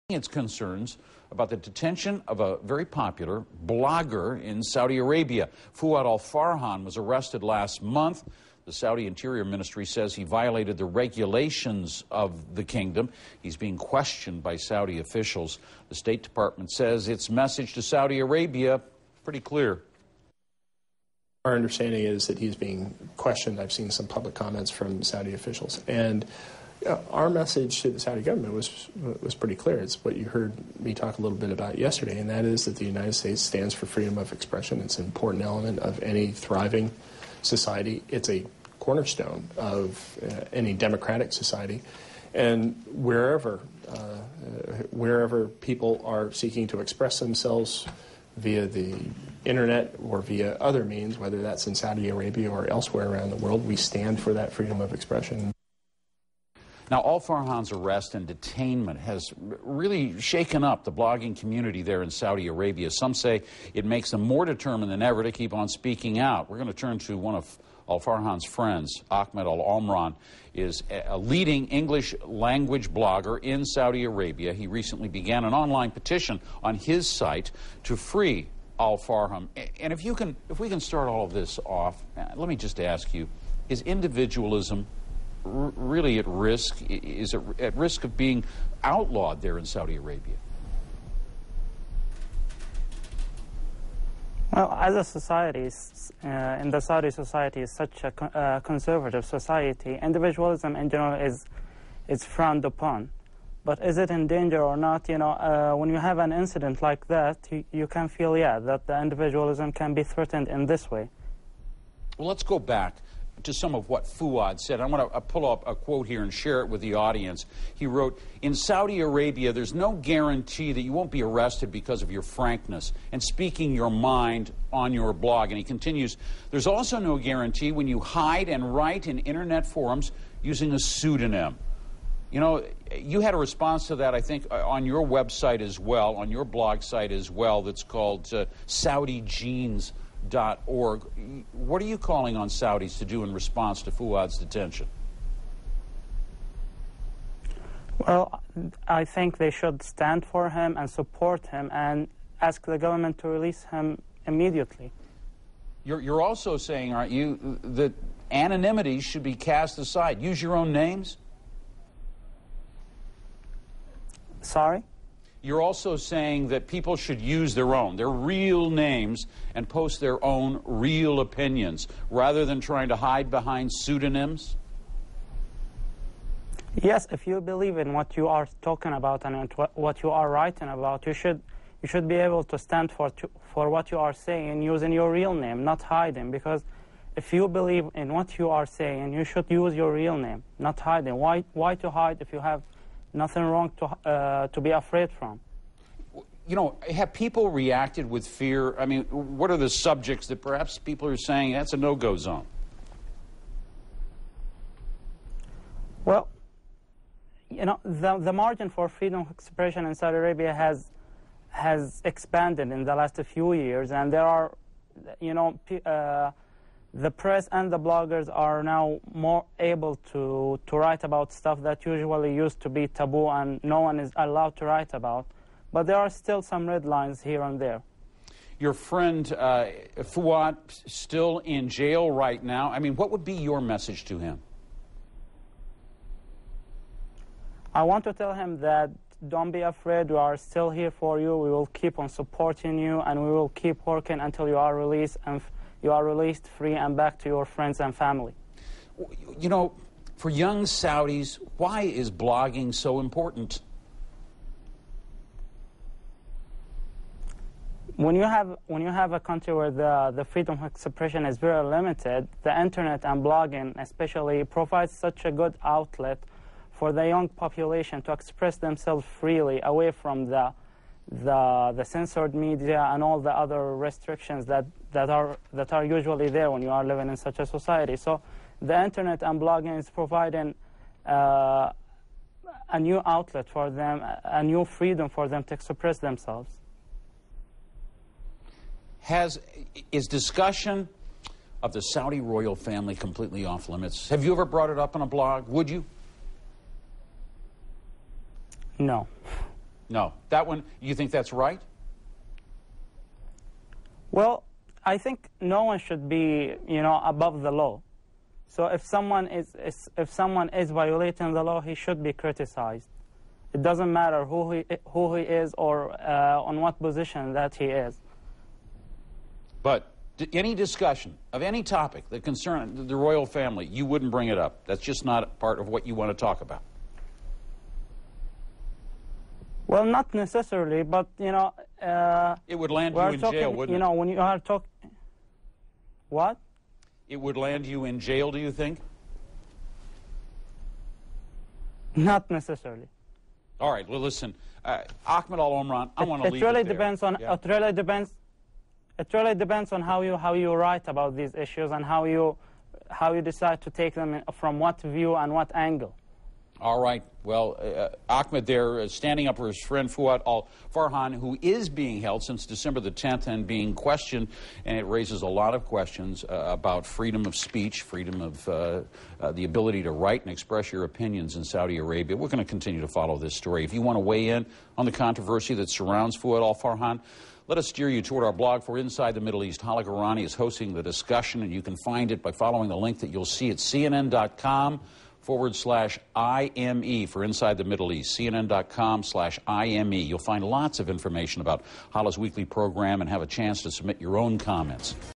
CNN on Fouad's Arrest
My interview on CNN about Fouad's case.